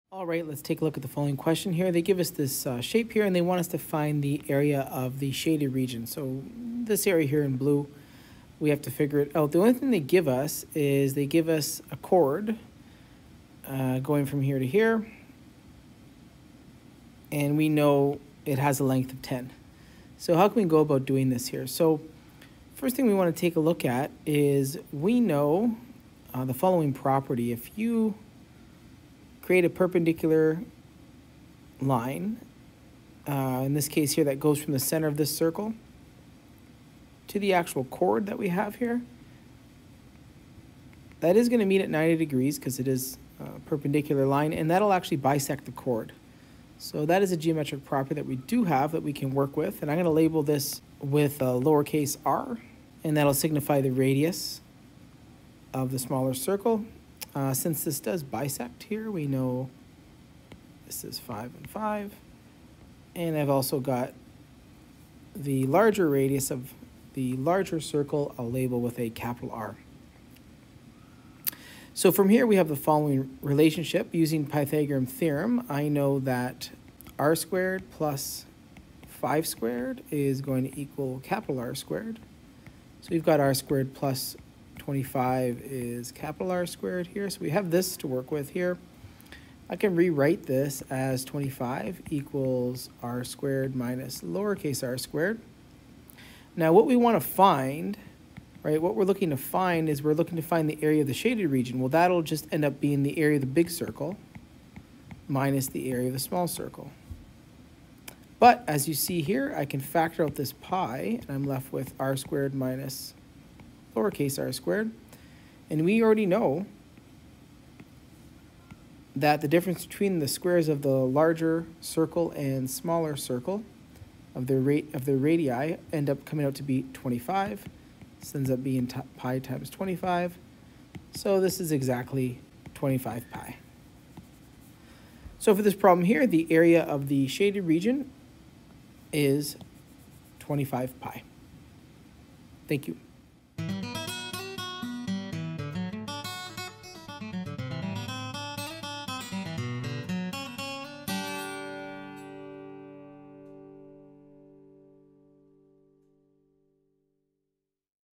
Math Competition Questions - Find the Area of the shaded region
In this math competition question we look to find the area of the shaded region.